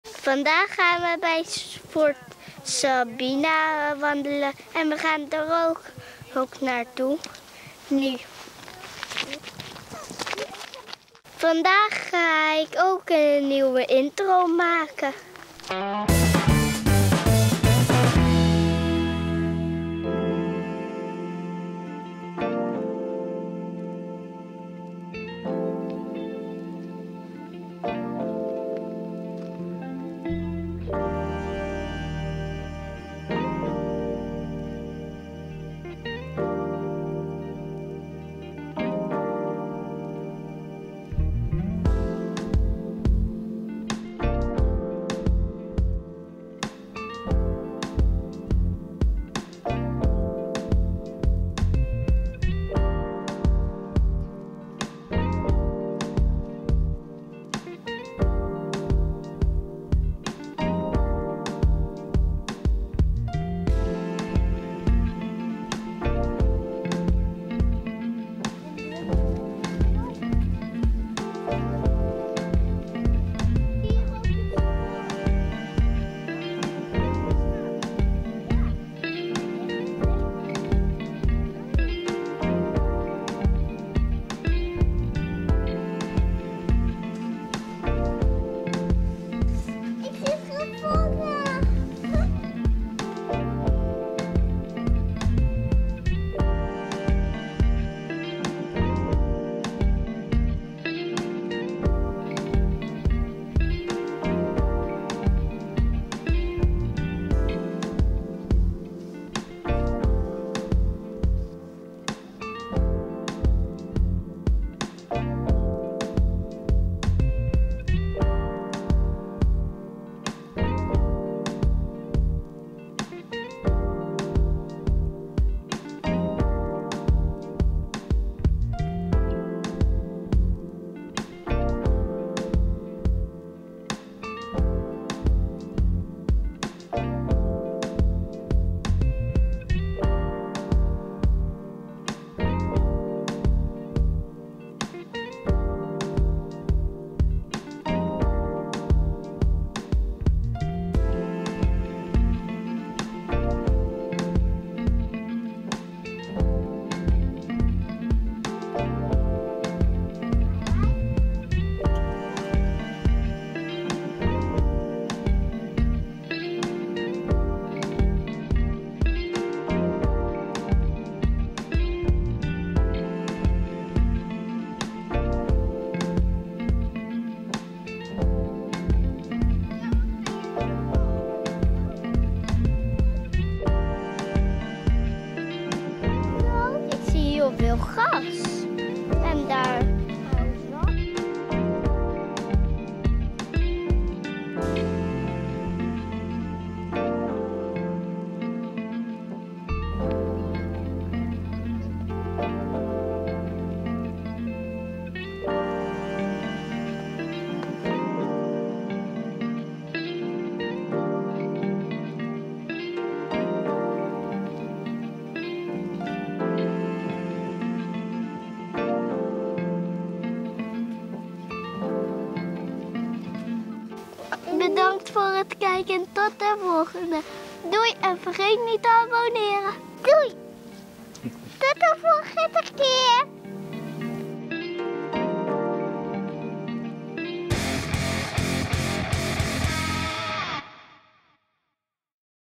Wandelen🥾 op Fort Sabina🏰 - Vlog #24
We zijn gaan wandelen bij Fort Sabina in Heijningen en hebben een nieuw intro en outro filmpje gemaakt!

Fort Sabina is in 1811 gebouwd en heeft al diverse oorlogen doorstaan. Nu kun je er rondwandelen en is het er lekker rustig. Het fort is onderdeel van de Zuidwaterlinie.